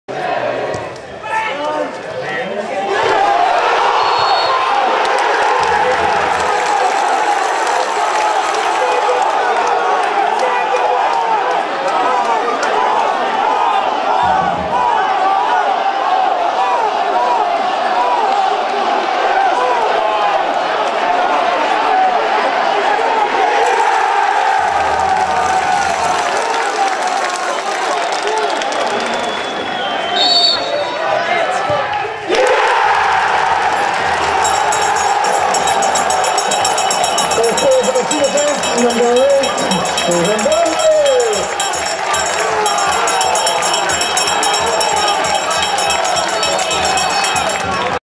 Goal: Stephen Bradley
Stephen Bradley scores the equalizing goal against Bohemians at Richmond Park.